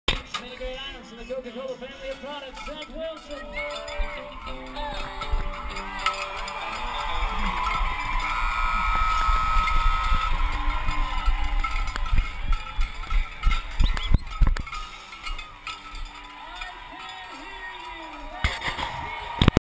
Seth Wilson Winning the mini-bull riding on crooked nose
Seth's winning ride 6-27